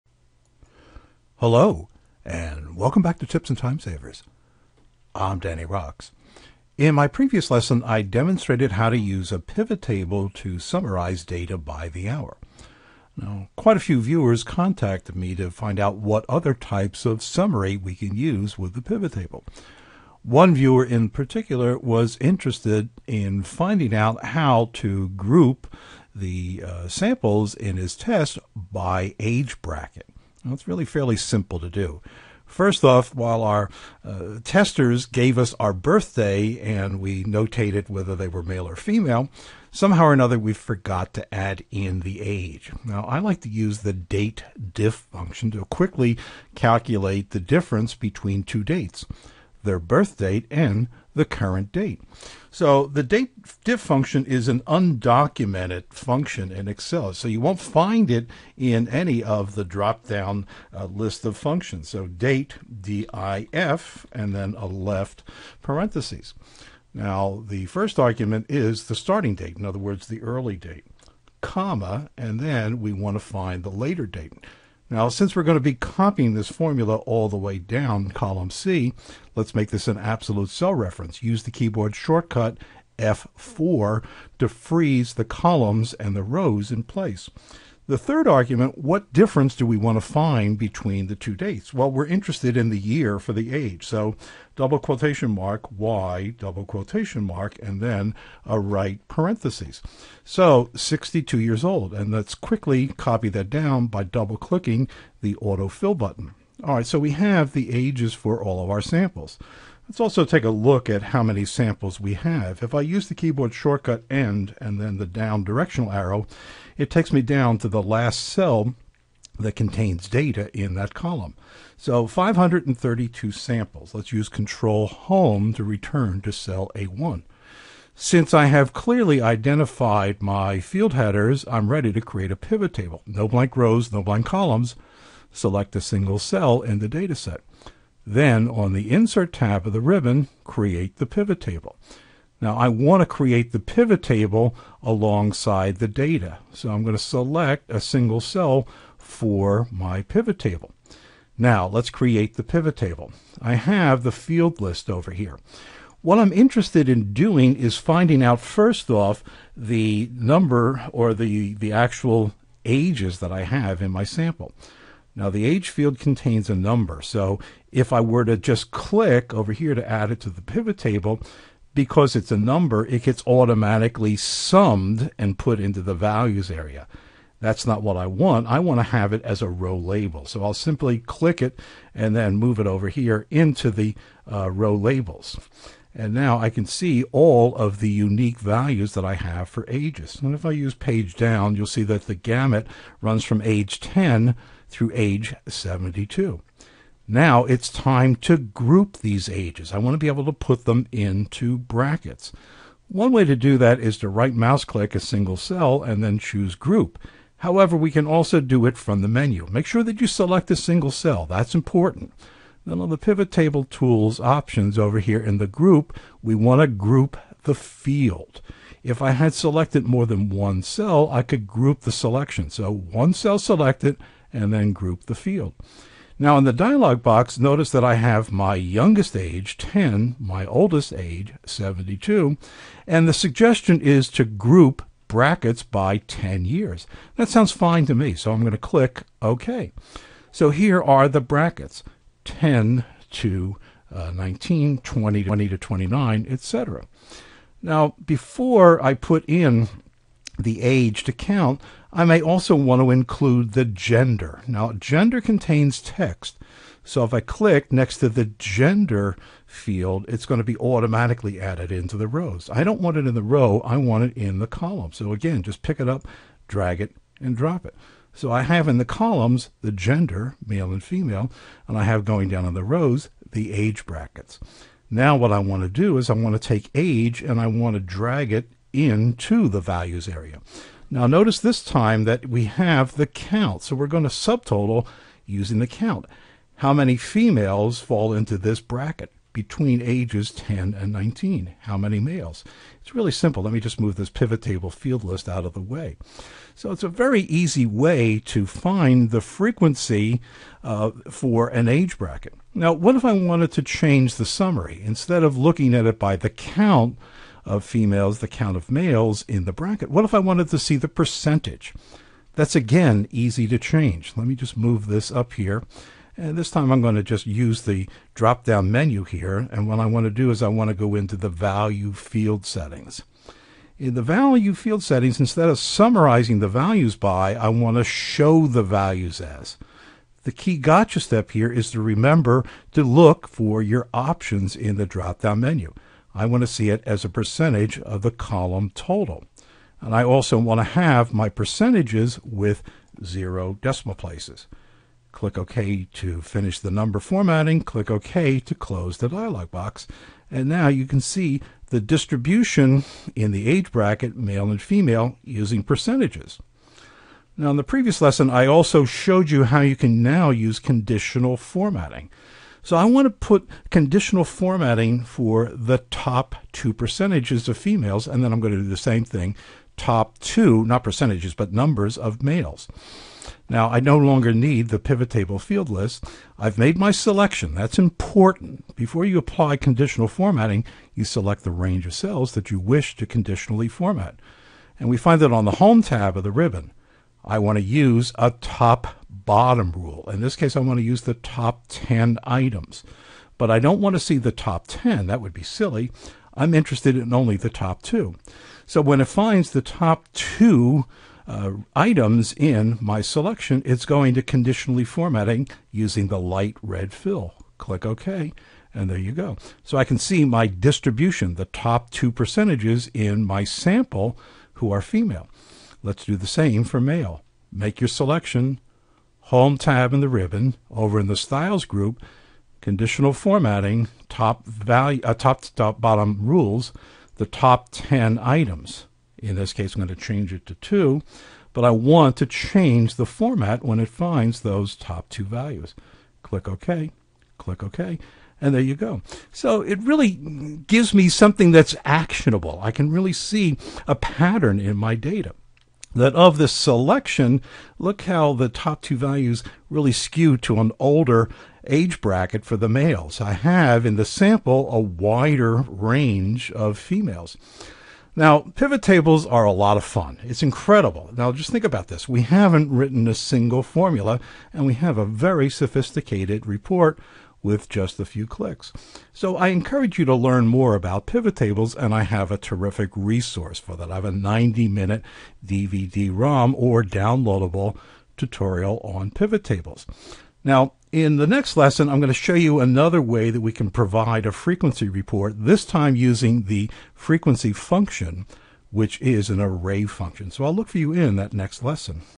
Use an Excel Pivot Table to Group Data by Age Bracket
Several viewers asked me to demonstrate some other ways to -Group a Field- in a Pivot Table. In this tutorial, I show you how to create a Frequency Report by Age Bracket.

Before I start the Pivot Table Demonstration, I show you how to calculate someone's age using the -undocumented- =DATEDIF() Function in Excel.

In the Pivot Table, I change the result from -Summarize Values By- to the -Show Vales As- setting. Finally, I apply Conditional Formatting to the Pivot Table.

I invite you learn more about my Pivot Table Video Tutorials.